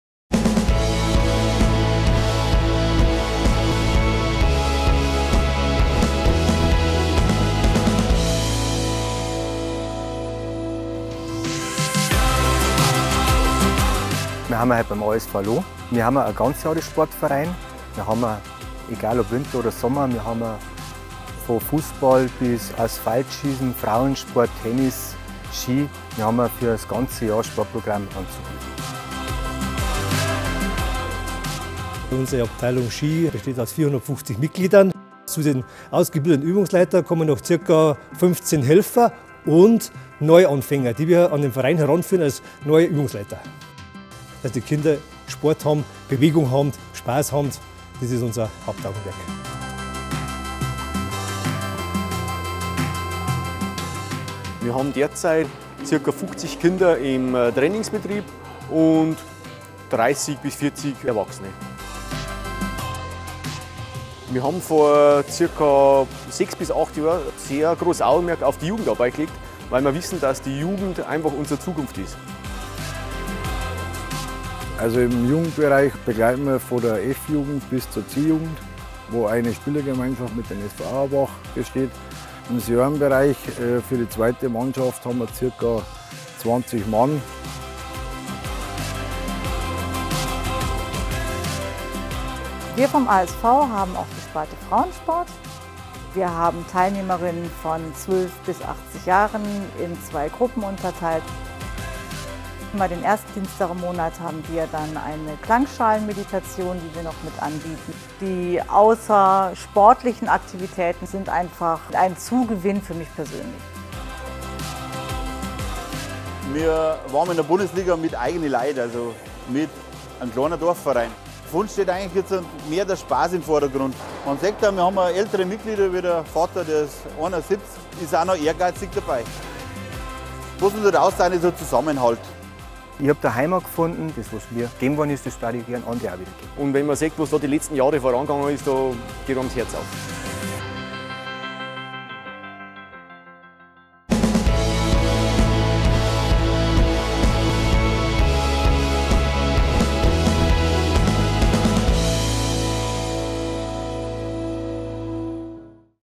ASV Loh - Sportverein des Monats Februar 2023
Sport und Spaß das ganze Jahr über! Dafür steht unser Sportverein des Monats, der ASV Loh e.V. mit seinen fünf Sparten Ski, Tennis, Fußball, Frauensport und Eisstock.
Der Zusammenhalt und die außersportlichen Aktivitäten machen den Verein für die Mitglieder zur sportlichen Heimat und sind ein Zugewinn fü jeden persönlich.